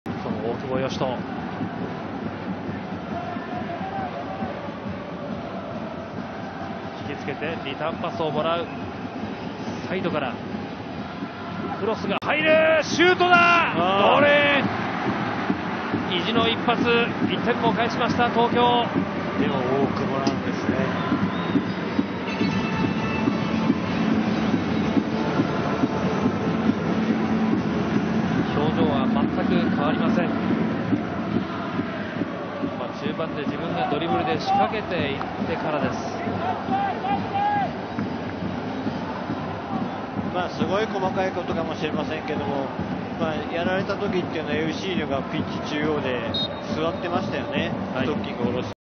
【公式】ゴール動画：大久保 嘉人（FC東京）90+1分 ＦＣ東京vs川崎フロンターレ ＪリーグYBCルヴァンカップ 準々決勝 第2戦 2017/9/3
大久保 嘉人（FC東京）が左後方から供給された緩やかなクロスを右足で流し込む！
2017年9月3日（日）に行われたＪリーグYBCルヴァンカップ 準々決勝 第2戦 FC東京vs川崎Ｆのゴール動画です。